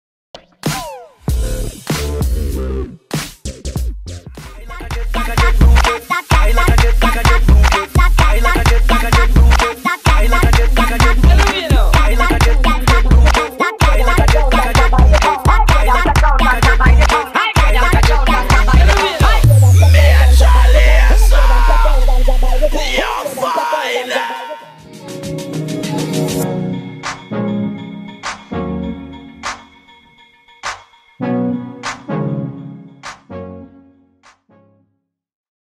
Activación Rompe trafico - Real Plaza - Activa Btl - 2016
Activación rompe trafico para promocionar el Shopping Day del Real Plaza.
Acción que realizamos en Av. La Cultura, a la altura del Real Plaza.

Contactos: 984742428